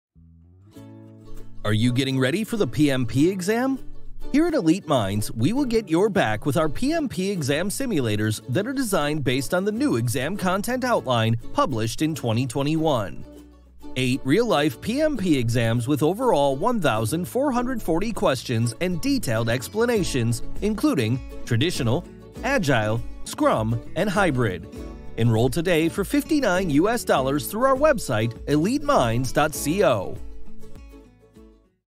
PMP® Certification Exam Simulator | PMP® 8 Mock Exams | 1440 PMP® Questions | Elite Minds
The structure of this PMP® simulator exam questions is designed as per the new format of the PMP exam (2022) by PMI which had become effective from 2nd Jan 2021 (50% Agile and 50% Predictive/Hybrid). This simulator includes questions from the newly published PMBOK 7th Edition by PMI as well. Situational and scenario-based questions will form the core of the PMP® certification exam.

About 70-80% of the total number of questions in the PMP® exams will be situational questions. Hence, it is very important that you develop your skill of solving these type of questions in the context of the revised syllabus of the PMP® Exam by PMI.

With The Elite Minds  PMP® Exam Simulator you’ll gain access to eight full exam samples that mimic the real test environment as closely as possible, down to the features you’ll see on the screen. Practicing the simulator's highly realistic questions will boost both your competency and confidence in taking the  PMP® exam.